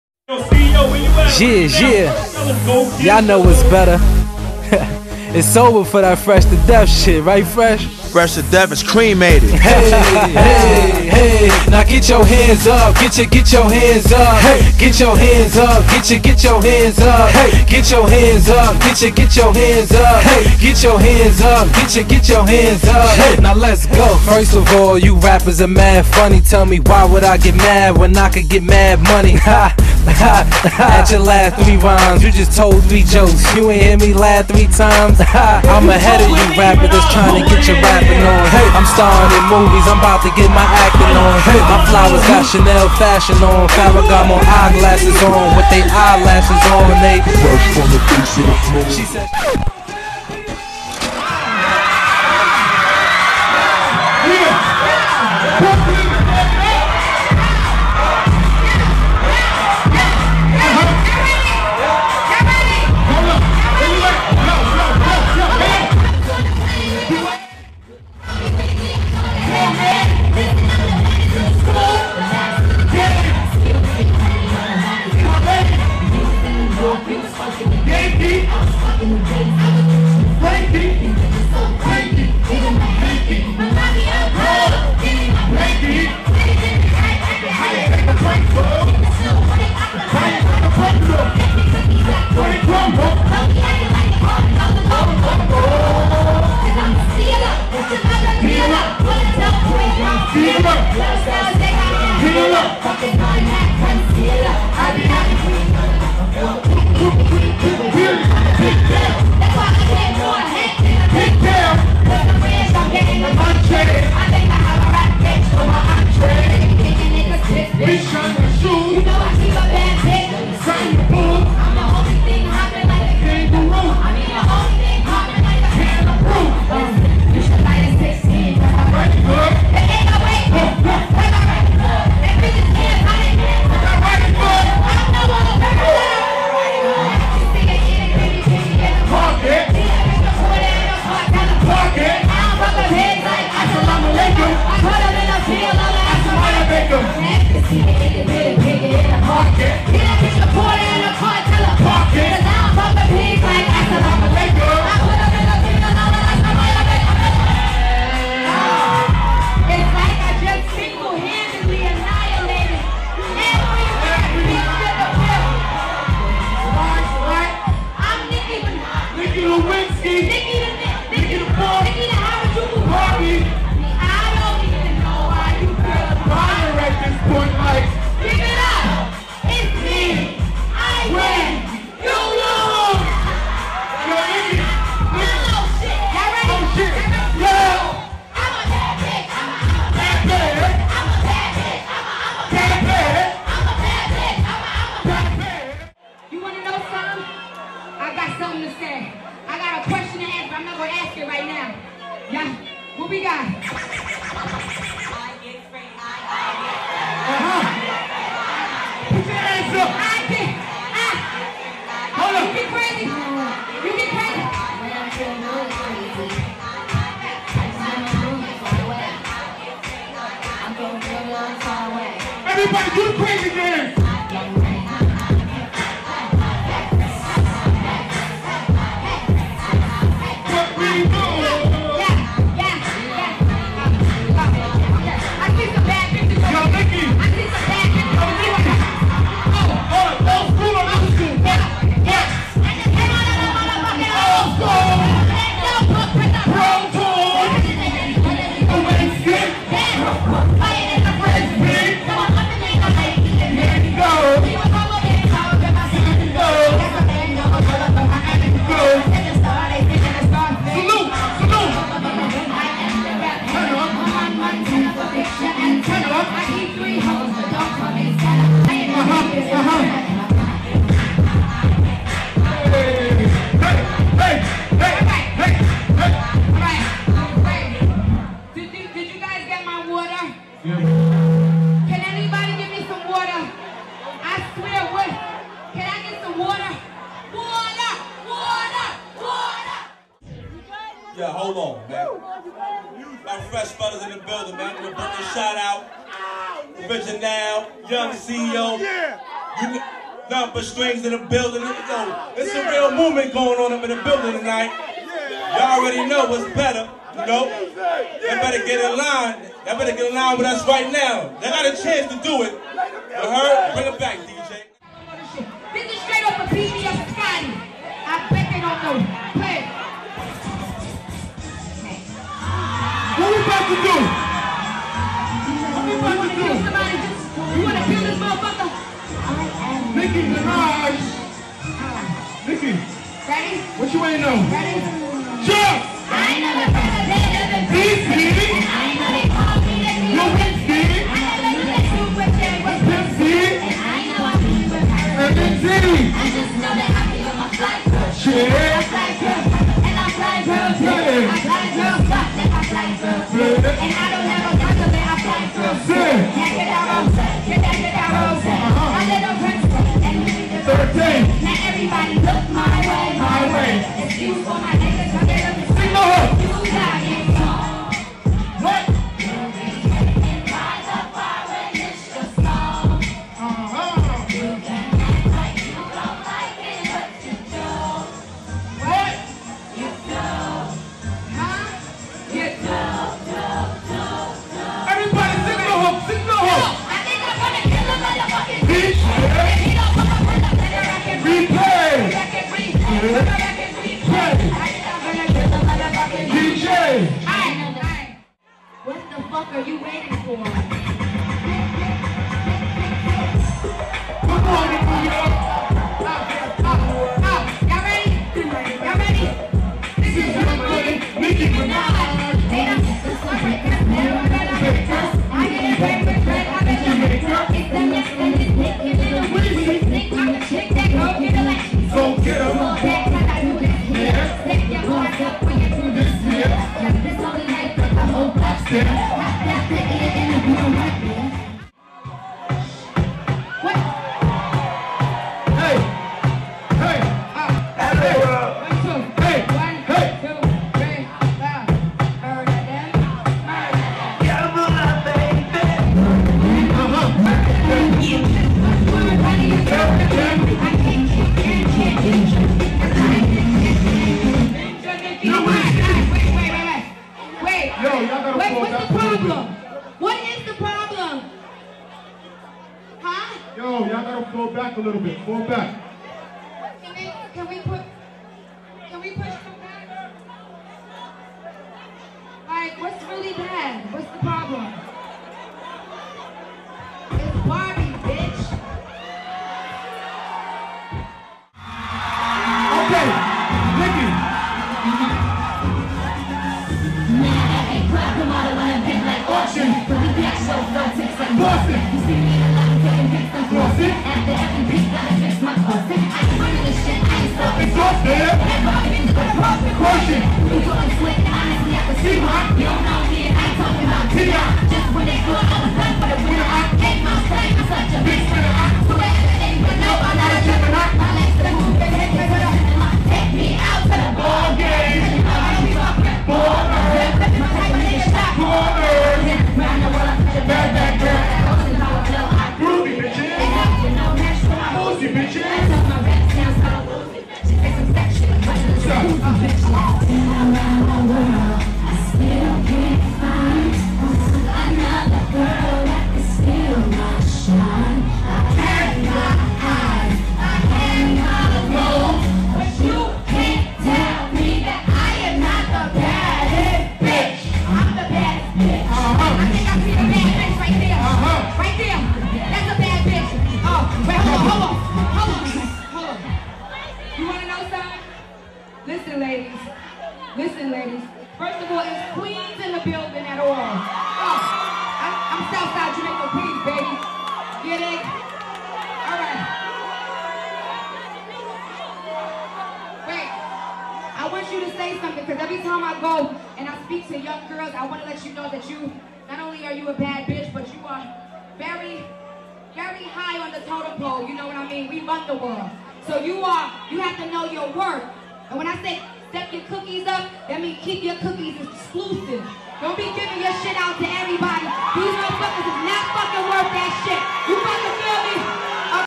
THROWBACK NIKKI MINAJ LIVE IN BK (POLOVISION) PT 1
FRESH FELLAZ SHOW AT DJUMBALA FEAUTRING NIKKI MINAJ AND RED CAFE.
THIS IS NIKKI MINAJ FIRT PART OF THE SHOW.